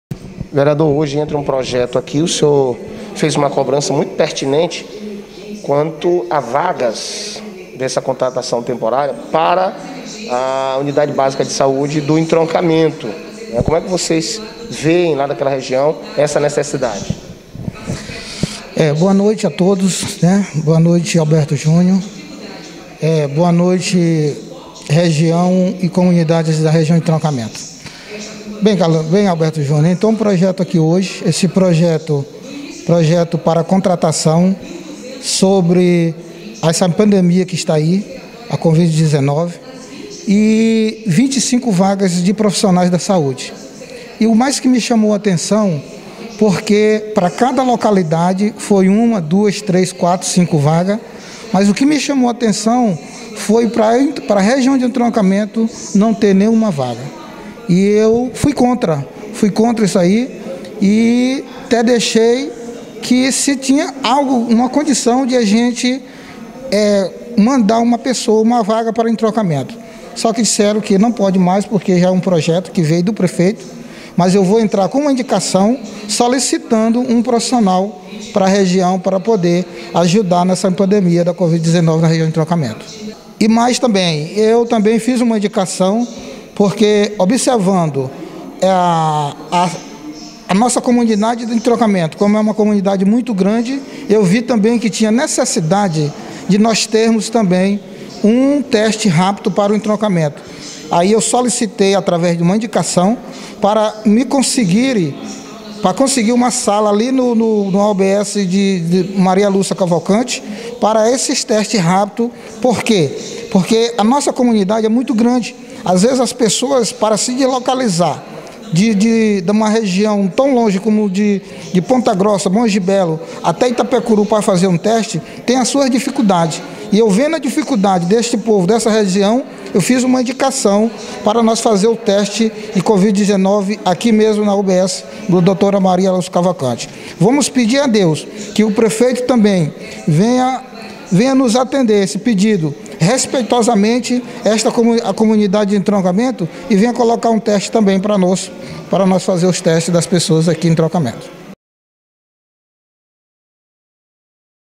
Vereador Wilsom Aires cobra vagas de contratação temporária para Entroncamento
O vereador Wilsom Aires (PSD) usou a tribuna para cobrar inclusão de vagas para contratação temporária de profissionais da saúde no povoado Entroncamento e apresentou indicação solicitando um centro de atendimento a pacientes com covid-19 naquela localidade para atender as mais de 20 comunidades adjacentes.